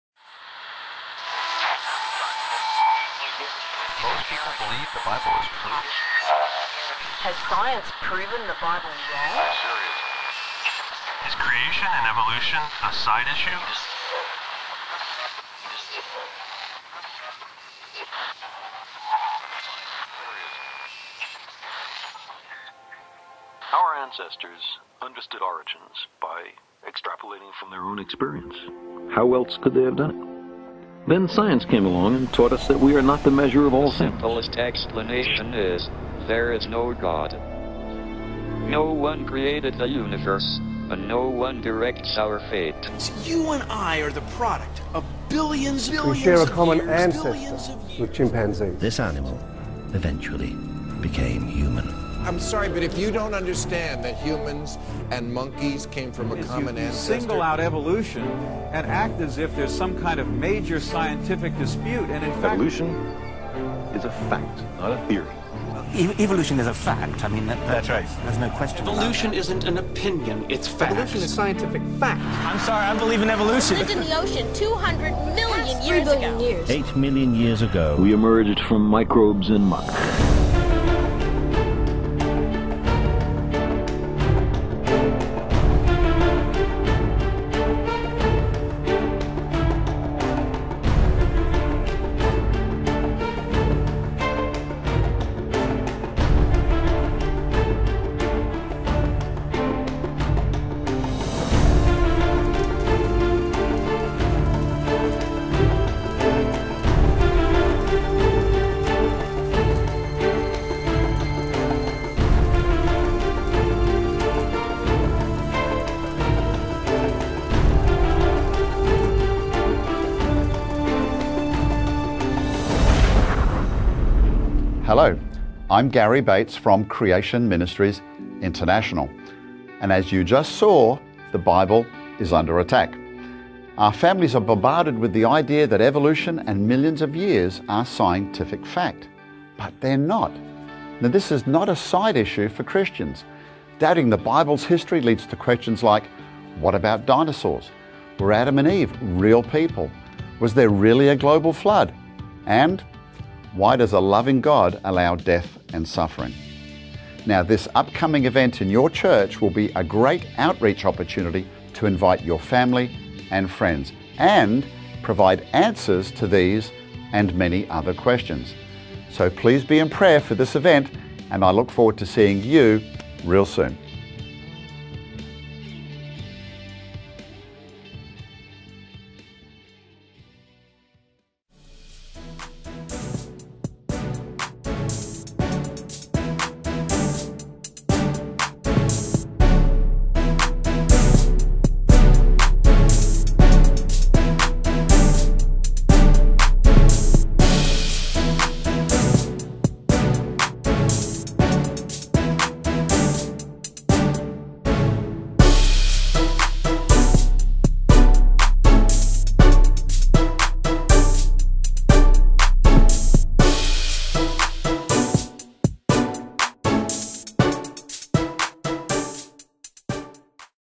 Creation Ministries International Coming to NFBC
Sunday March 8th we welcome Dr. Gary Bates from Creation Ministries International! He will be speaking on Dinosaurs at 9 am, Creation at 10:30 am, and the biblical perspective on the Alien phenomenon at 6 pm! We would love you to join us for any or all of these!